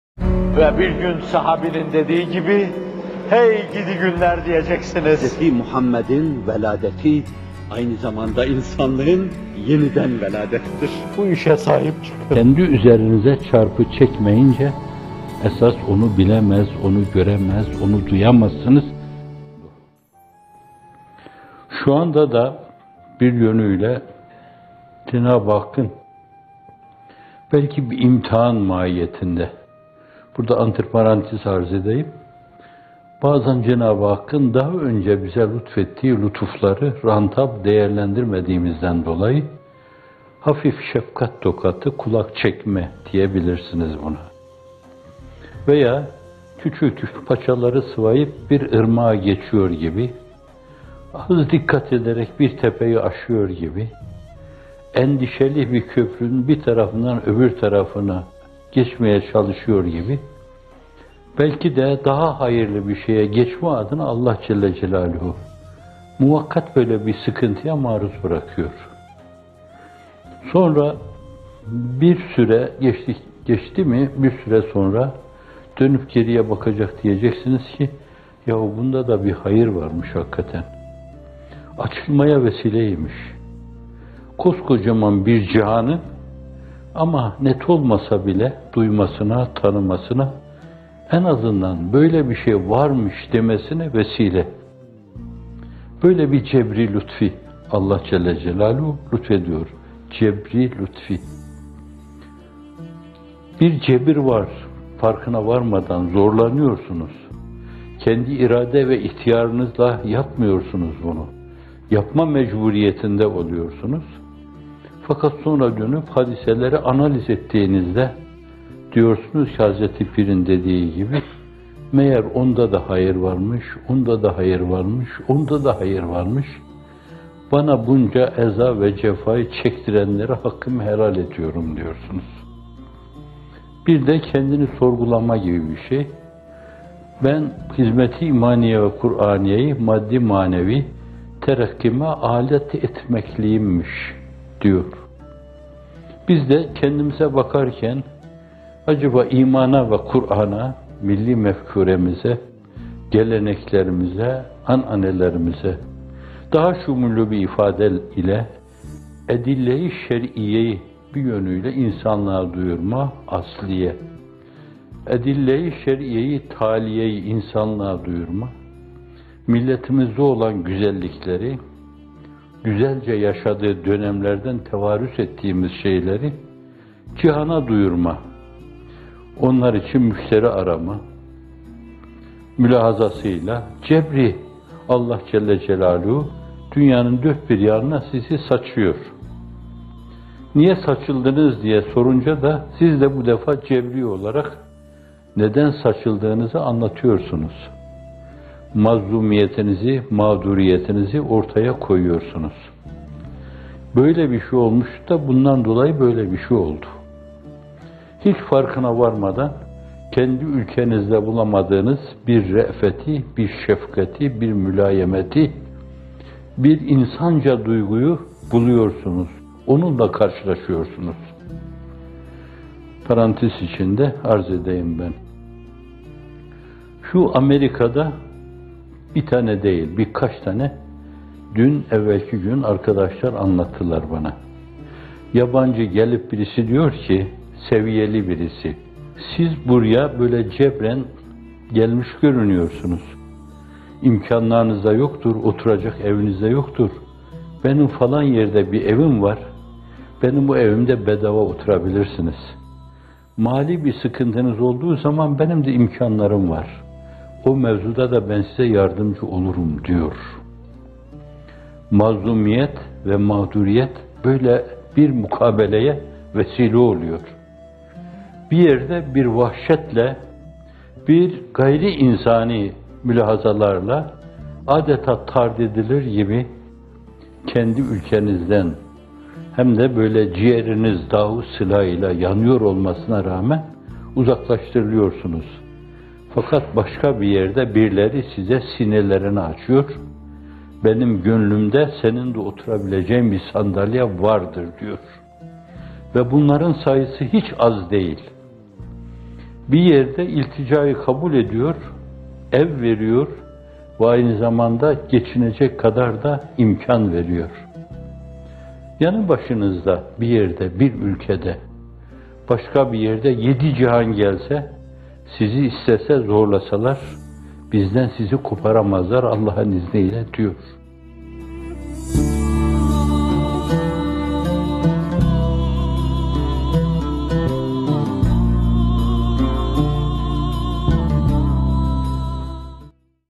“Yedi Cihan Gelse, Sizi İstese, Vermeyiz, Allah’ın İzniyle!” | Mizan | M. Fethullah Gülen Hocaefendi
Bu video 01/01/2017 tarihinde yayınlanan " CEBR-İ LÜTFÎ VE HASANÎ RUH" isimli bamtelinden alınmıştır.  Tamamı burada

Bugün maruz kaldığımız bela ve musibetlerin de pek çok cebr-i lütfî yanı olduğu muhakkaktır; bütün cihanın Hizmet’i duyup tanıması bu lütuflardan biri olsa gerektir.

Şu anda da -bir yönüyle- Cenâb-ı Hakk’ın belki bir imtihanı mahiyetinde benzer hadiselere şahit oluyoruz. Burada antrparantez arz edeyim: Bazen Cenâb-ı Hakk’ın daha önce bize lütfettiği ihsanları rantabl değerlendirmediğimizden dolayı hafif şefkat tokadı yiyoruz; kulak çekme de diyebilirsiniz buna. Veya paçaları sıvayıp küçük bir ırmağı geçiyor gibi.. az dikkat ederek bir tepeyi aşıyor gibi.. endişeli bir köprünün bir tarafından öbür tarafına geçmeye çalışıyor gibi… Belki de daha hayırlı bir şeye geçme adına, Allah (celle celâluhu) muvakkat bir sıkıntıya maruz bırakıyor. Bir süre geçti mi, bir süre sonra dönüp geriye bakacak, diyeceksiniz ki, “Yahu bunda da bir hayır varmış hakikaten. Açılmaya vesile imiş. Koskocaman bir cihanın -net olmasa bile- duymasına, tanımasına, en azından ‘Böyle bir şey varmış!’ demesine vesile imiş.” Böyle bir cebr-i lütfî. Allah (celle celâluhu) lütfediyor.

Cebr-i lütfî… Bir cebir var, farkına varmadan zorlanıyorsunuz; kendi irade ve ihtiyarınızla yapmıyorsunuz onu, yapma mecburiyetinde oluyorsunuz. Fakat sonra dönüp hadiseleri analiz ettiğinizde, diyorsunuz ki, Hazreti Pîr’in dediği gibi, “Meğer onda da hayır varmış, onda da hayır varmış, onda da hayır varmış!..” “Bana bunca ezâ ve cefayı çektirenlere hakkımı helal ediyorum!” diyorsunuz. Bir de kendini sorgulama gibi bir şey; “…Hizmet-i imaniye ve Kur’anîyeyi maddî-manevî terakkime âlet etmekliğimmiş!” diyor.

Biz de kendimize ve hadiselere bakarken, bu mülahazayı esas almalıyız. Acaba imana, Kur’an’a, millî mefkûremize, geleneklerimize ve an’anelerimize hizmet ettirmek için mi sizi dünyanın dört bir yanına saçıyor?!. Evet, daha şümullü bir ifade ile, “edille-i şer’iyye-yi asliye” ve “edille-i şer’iyye-i tâliye”yi insanlığa duyurma, milletimizde olan güzellikleri, güzelce yaşadığı dönemlerden tevârüs ettiğimiz şeyleri cihana duyurma, onlar için müşteri arama maksadıyla, Allah (celle celâluhu) cebrî olarak dünyanın dört bir yanına sizi saçıyor. “Niye saçıldınız?” diye sorulunca, siz de bu defa cebrî olarak neden saçıldığınızı anlatıyorsunuz. Mazlumiyetinizi, mağduriyetinizi ortaya koyuyorsunuz; “Böyle bir şey olmuş da bundan dolayı böyle bir şey oldu!..” Hiç farkına varmadan, kendi ülkenizde bulamadığınız bir re’feti, bir şefkati, bir mülâyemeti, bir insanca duyguyu buluyorsunuz, onunla karşılaşıyorsunuz.

Parantez içinde arz edeyim: Şu Amerika’da, bir tane de değil birkaç tane, dün ve evvelki gün, arkadaşlar anlattılar. Yabancı birisi gelip diyor ki, seviyeli birisi, “Siz, buraya cebren gelmiş görünüyorsunuz. İmkânlarınız da yoktur, oturacak eviniz de yoktur. Benim falan yerde bir evim var, o evde bedava oturabilirsiniz! Malî bir sıkıntınız olduğu zaman, imkânlarım da var, o mevzuda da size yardımcı olurum!..” diyor. Mazlumiyet ve mağduriyet, böyle bir mukabeleye vesile oluyor. Bir yerde bir vahşetle, gayr-ı insanî mülahazalarla, âdetâ tardedilir gibi kendi ülkenizden, hem de ciğeriniz dâussıla ile yanıyor olmasına rağmen, uzaklaştırılıyorsunuz. Fakat başka bir yerde birileri size sinelerini açıyor, “Benim gönlümde senin de oturabileceğin bir sandalye vardır!” diyorlar. Ve bunların sayısı hiç az değil. Bir yerde ilticayı kabul ediyor, ev veriyor ve aynı zamanda geçinecek kadar da imkân veriyorlar; yanı başınızda bir yerde, bir ülkede. Başka bir yerde “Yedi cihan gelse, sizi istese, zorlasalar, bizden sizi koparamazlar, Allah’ın izniyle!” diyorlar.

►Bizimle paylaşmak istediğiniz haberler için e-posta adresimiz 👉